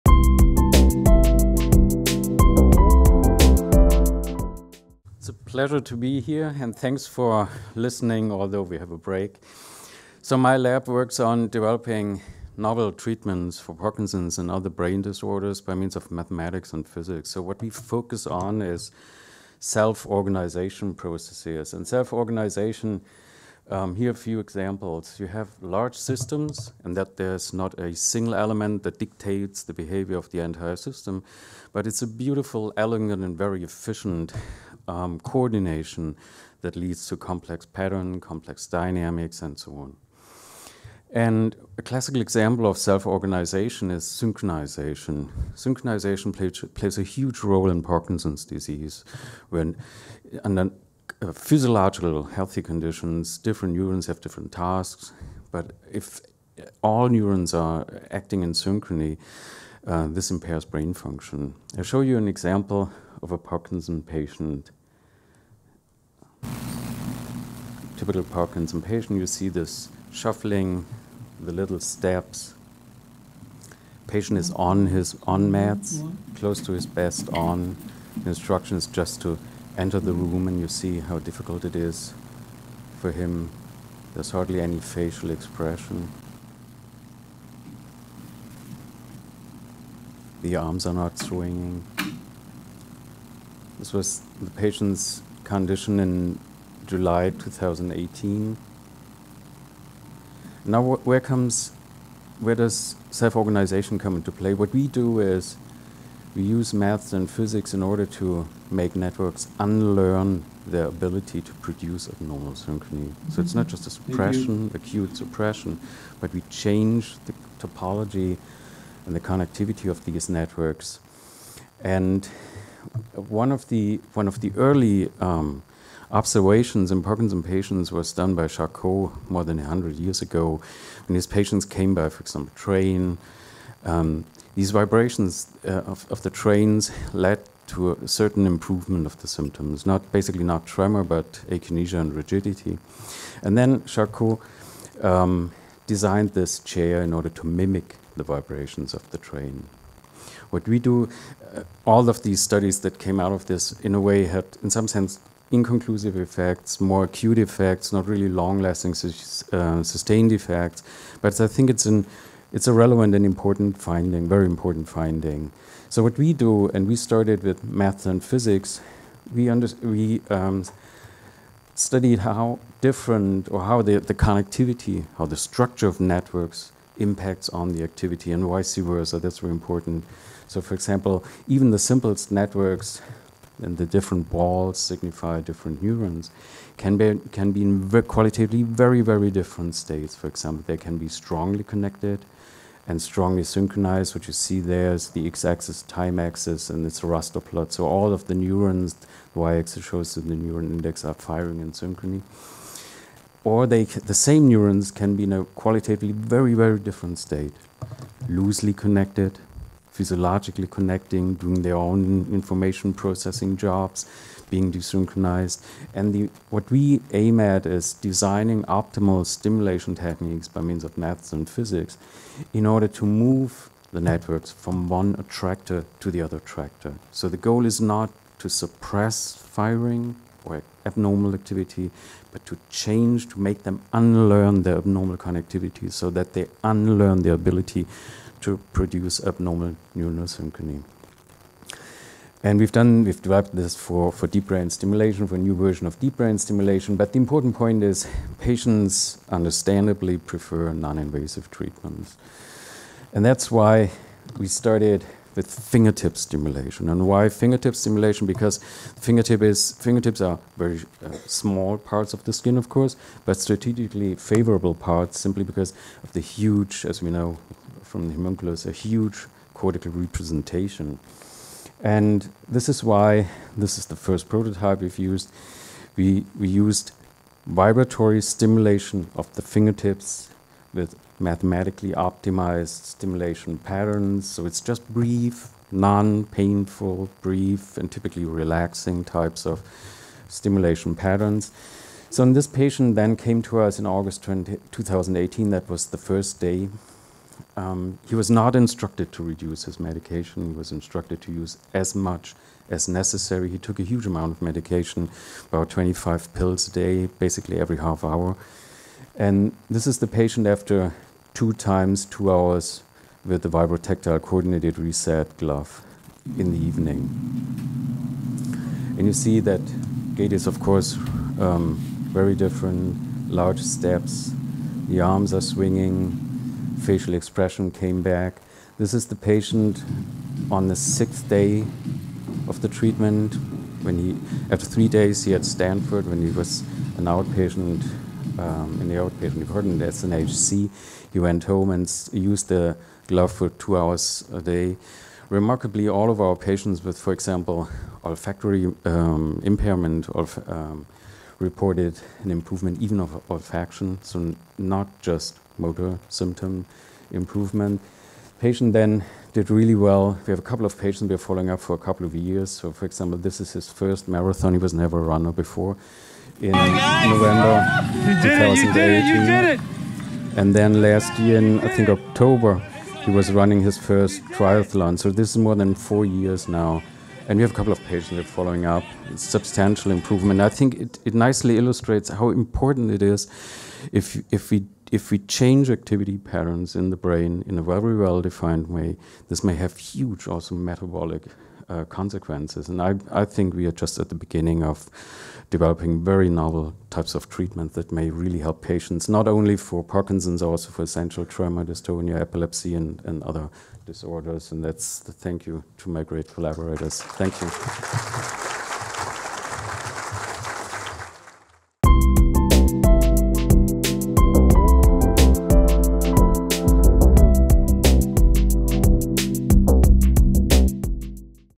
Stanford Medicine Alumni Day 2023 - Vibratory Glove: A New Way To Treat Parkinson’s Disease
PETER A. TASS, MD, PHD

Dr. Tass investigates and develops neuromodulation techniques for understanding and treating neurologic conditions such as Parkinson’s disease, epilepsy, dysfunction following stroke, and tinnitus. His work creates therapeutic procedures by means of comprehensive computational neuroscience studies and advanced data analysis techniques. From this research, he has pioneered a neuromodulation approach that employs dynamic self-organization, plasticity, and other neuromodulation principles to produce sustained improvement after stimulation. Dr. Tass received his MD from the Universities of Ulm and Heidelberg in Germany and his master’s degree in mathematics and PhD in physics from the University of Stuttgart in Germany.